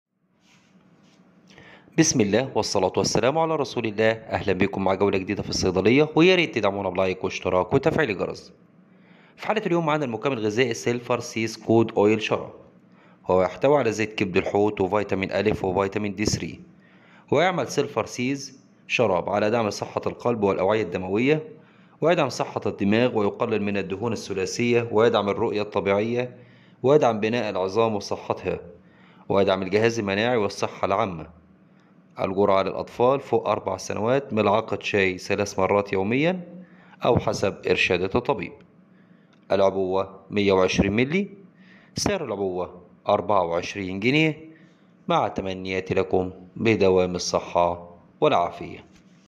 شراب راااائع رخيص جداً زيت كبد الحوت مع فيتامين A & فيتامين D3
شراب رخيص جداً زيت كبد الحوت مع فيتامين A & فيتامين D3
اسم الصنف
سيلفر سيز كود اويل شراب
SILVER SEAS COD LIVER SYRUP
ويحتوي علي
Each 5 ml contains:
Cod liver oil      150 mg
(vitamin A 1000 IU/gm) 150 IU
(vitamin D3 100 IU/gm)  15 IU
Retinol palmitate (1.5 M.I.U/gm) 0.583 mg
Equivalent to vit. A 874.5 IU)
Colecalciferol (4 M.I.U/gm) 0.0145 mg
Equivalent to vit. D3 58 IU)